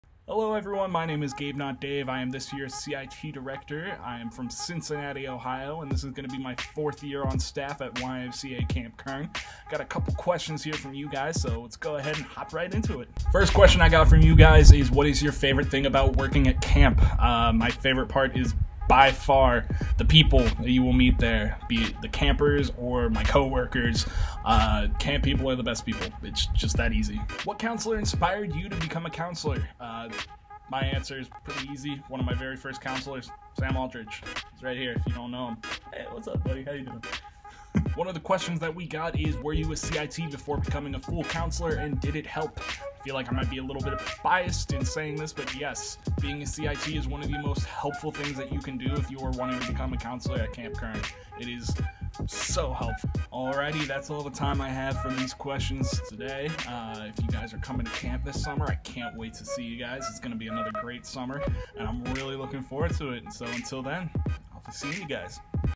Summer 2020 - Meet Gabe
Look, Gabe (not Dave) is back this summer! Find out what his favorite thing about camp is in this video!